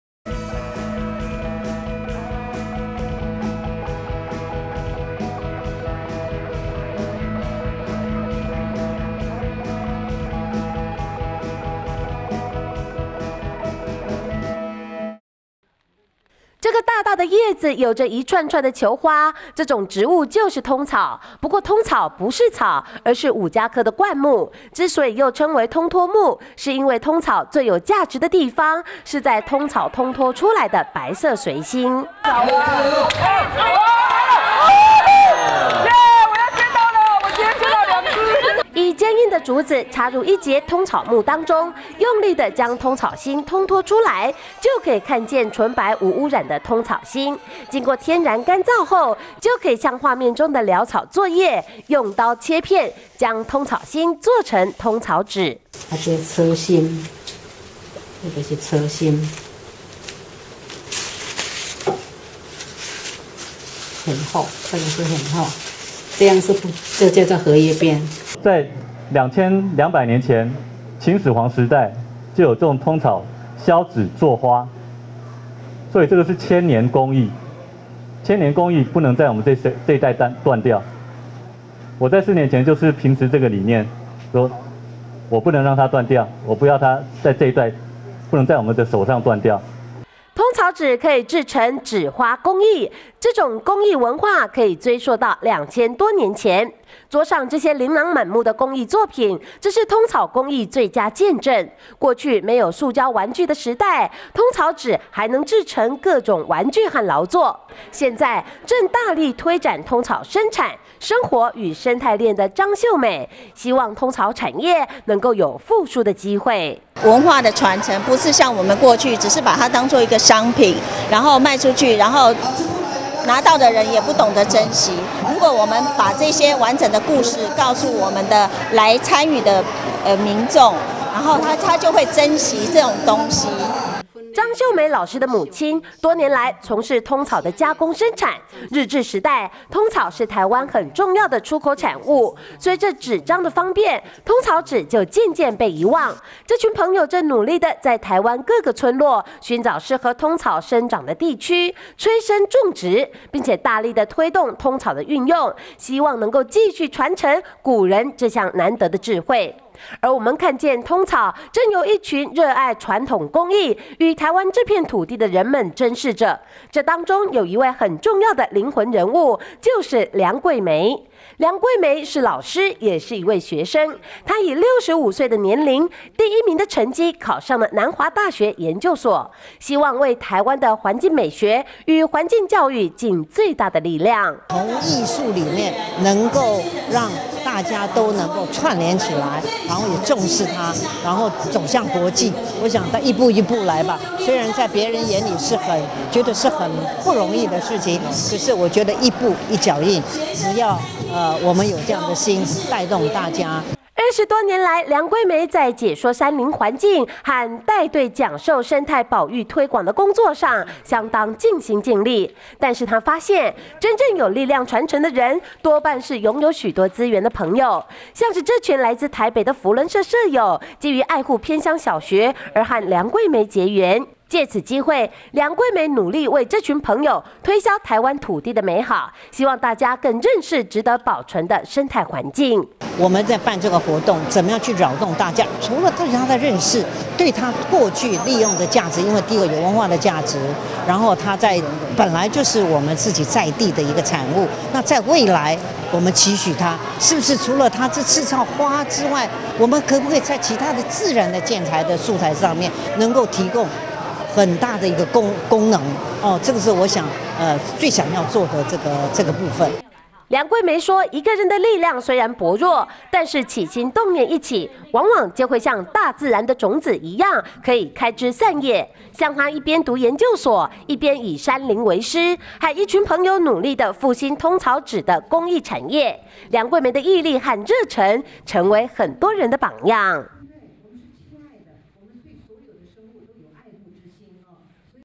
1011117梁貴梅張秀美發揚蓪草工藝生態鏈
1011117梁貴梅張秀美發揚蓪草工藝生態鏈
蓪草為五加科灌木 白色髓心高價值
通脫蓪草心 乾燥後切片作成蓪草紙
蓪草產業紙花工藝 張秀美大力推展
催生種植蓪草 傳承古人的蓪草智慧
65歲梁貴梅 第一名考上南華大學
解說山林環境 努力推銷台灣美好
梁貴梅以山林為師 毅力令人激賞
這個大大的葉子，有著一串串的球花，這種植物就是蓪草，不過蓪草不是草，而是五加科的灌木，之所以又稱為通脫木，是因為蓪草最有價值的地方是在蓪草通脫出來的白色髓心。
以堅硬的竹子，插入一節蓪草木當中，用力的將蓪草心通脫出來，就可以看見純白無污染的蓪草心，經過天然乾燥後，就可以像畫面中的撩草作業，用刀切片，將蓪草心，作成蓪草紙。
01 蓪草文化藝術工作室總監 陳建華
蓪草紙可以製成紙花工藝，這個紙藝文化可追溯到二千多年前。桌上這些琳瑯滿目的工藝作品，則是蓪草工藝最佳見證，過去，沒有塑膠玩具的時代，蓪草心還能製作成各種玩具和勞作。現在，正大力推展蓪草生產、生活與生態鏈的張秀美，希望蓪草產業能夠有復甦的機會。
02 蓪草工藝推動者 張秀美
張秀美老師的母親，多年來從事蓪草的加工生產，日治時代，蓪草是台灣很重要的出口產物，隨著紙張的方便，蓪草紙就漸漸被遺忘，這群朋友正努力的在台灣各個村落，尋找適合蓪草生長的地區，催生種植，並且大力的推動蓪草的運用，希望繼續傳承古人這項難得的智慧。
而我們看見蓪草正由一群熱愛傳統工藝，與台灣這片土地的人們珍視著，這當中有一位很重要的靈魂人物，就是梁貴梅，梁貴梅是老師，也是一位學生，她以六十五歲的年齡，第一名的成績，考上了南華大學研究所，希望為台灣的環境美學與環境教育，盡更大的力量。
03 南華大學研究生 梁貴梅
二十多年來，梁貴梅在解說山林環境，和帶隊講授生態保育推廣的工作上，相當盡心盡力，但是她發現，真正有力量傳承的人，多半是擁有許多資源的朋友，像是這群來自台北的扶輪社社友，基於愛護偏鄉小學而和梁貴梅結緣，藉此機會，梁貴梅努力為這群朋友推銷台灣土地的美好，希望大家更認識值得保存的生態環 境。
04 南華大學研究生 梁貴梅
梁貴梅說，一個人的力量雖然薄弱，但是起心動念一起，往往會像大自然的種子一樣，可以開枝散葉，像她一邊讀研究所，一邊以山林為師，和一群朋友努力的復興蓪草紙的工藝產業，梁貴梅的毅力和熱忱，成為很多人學習的榜樣。